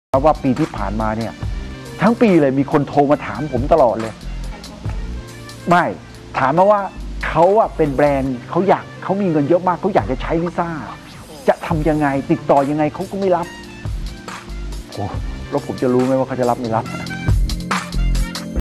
แบรนด์ไหนๆอยากจะมี LISA สำหรับแบรนด์ของพวกเขา
“ปีที่แล้ว ตลอดทั้งปี ฉันได้รับโทรศัพท์มากมายโดยบอกว่า “พวกเขามีเงินมากมาย และอยากจะมี LISA สำหรับแบรนด์ของพวกเขาด้วย จะทำอย่างไรและจะติดต่อ YG ได้อย่างไร?. พวกเขาไม่เคยรับสาย….+